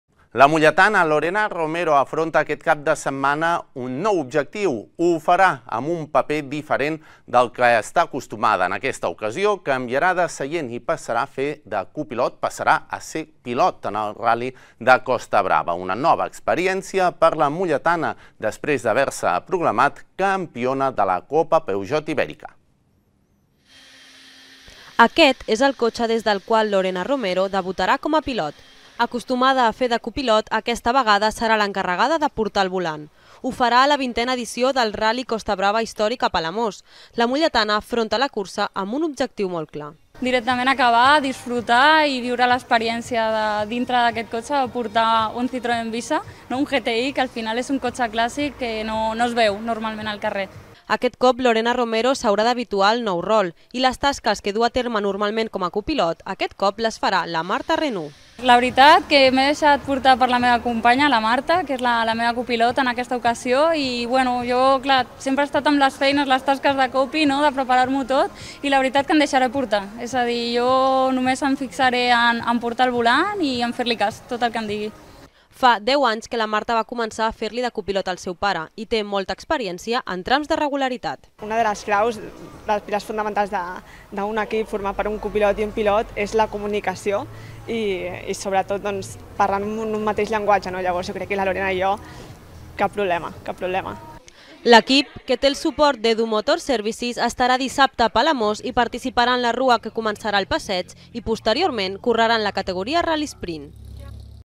Lorena Romero debutarà com a pilot en el Rally Costa Brava Històric
La molletana Lorena Romero afronta aquest cap de setmana un nou repte, i ho farà amb un paper diferent del qual està acostumada. En aquesta ocasió, canviarà de seient i passarà de fer de copilot a pilot en el Rally de Costa Brava. Una nova experiència per a la molletana després d'haver-se proclamat campiona de la Copa Peugeot Ibèrica.

Aquest cop, Lorena Romero s'haurà d'habituar al nou rol, i les tasques que duu a terme normalment com a copilot, aquest cop les farà la Marta Renú. Fa 10 anys que la Marta va començar a fer-li de copilot al seu pare i té molta experiència en trams de regularitat.

L'equip, que té el suport d'EDU Motor Services, estarà dissabte a Palamós i participarà en la rua que començarà al passeig. Posteriorment correrà en la categoria 'rallysprint'.